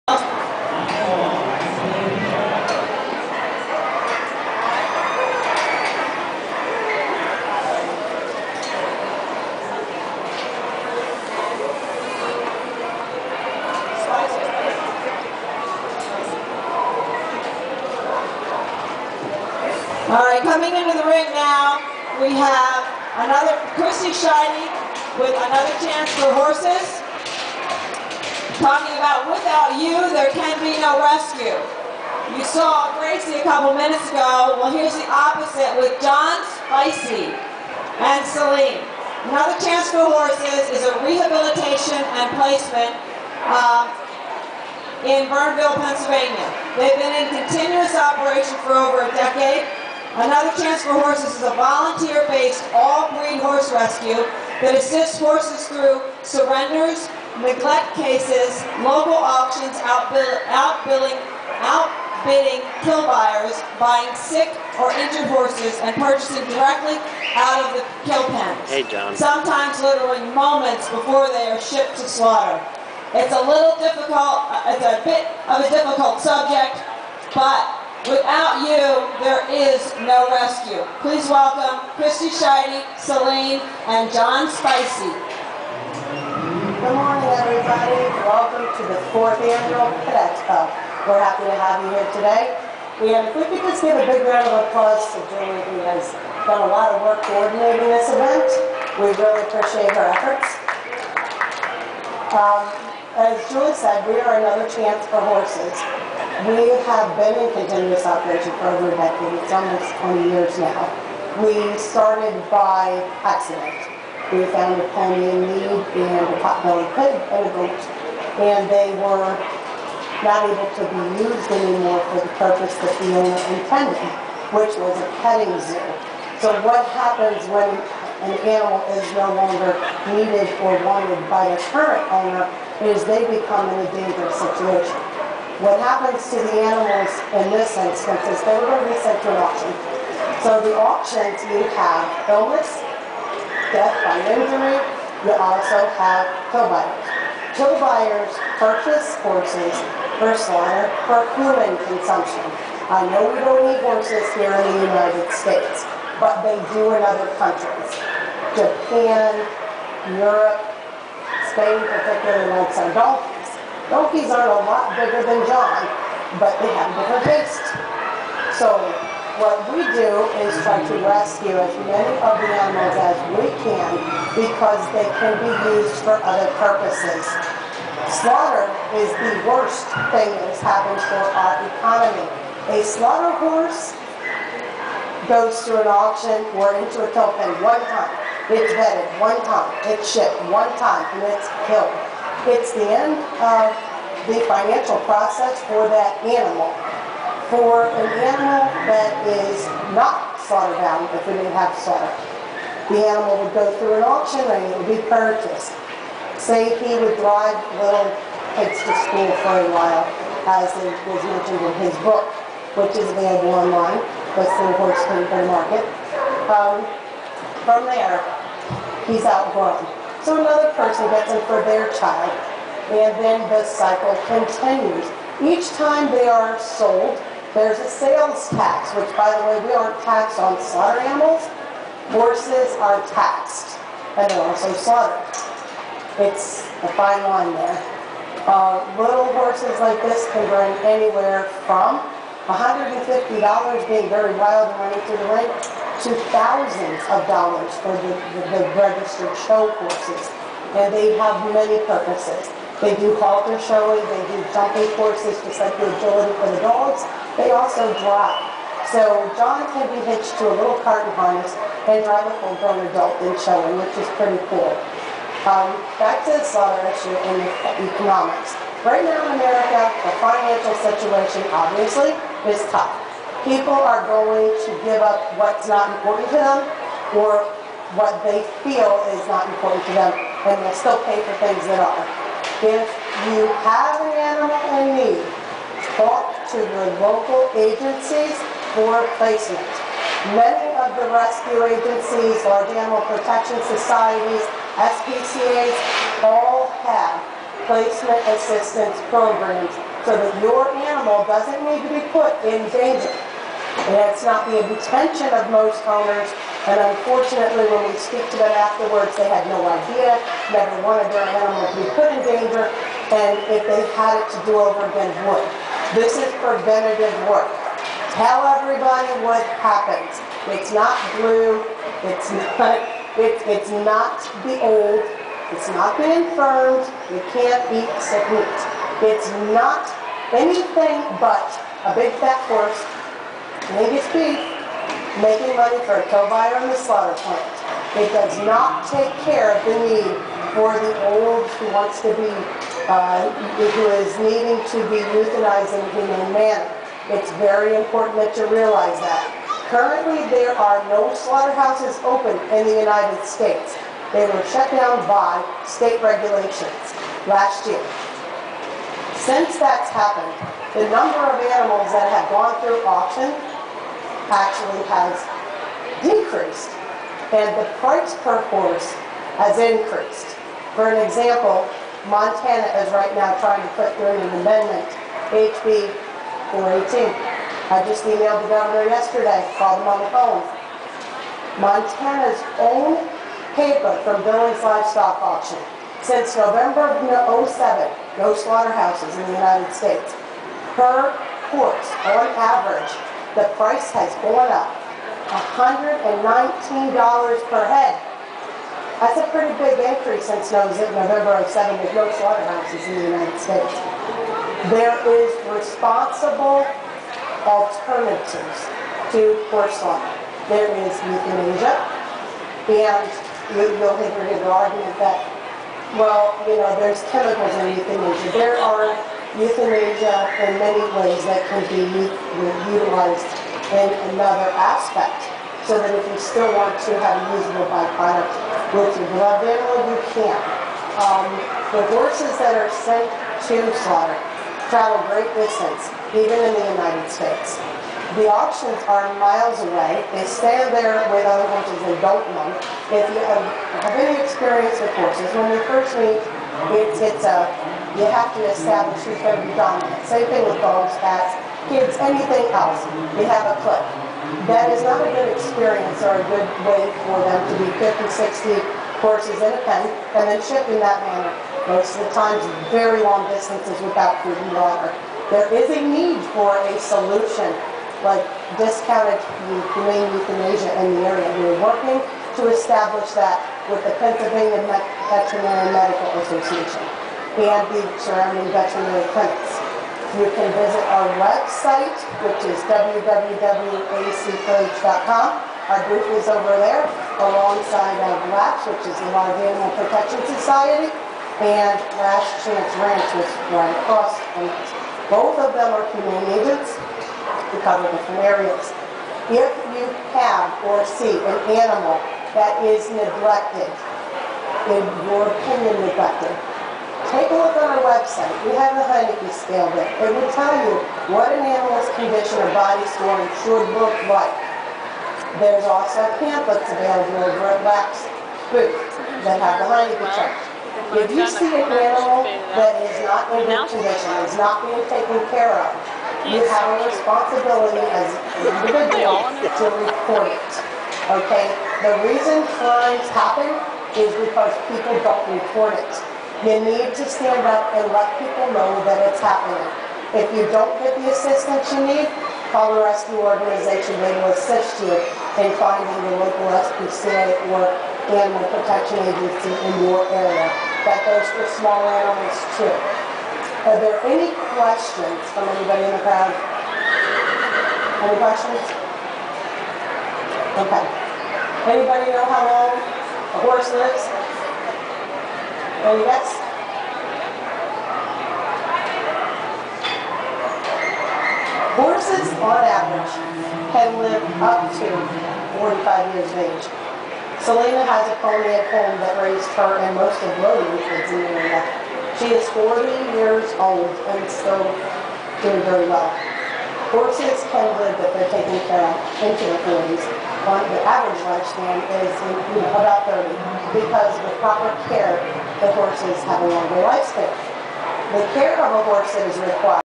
Christy Sheidy speaking at Pet Expo Reading PA The Largest Pet Expo on the East Coast.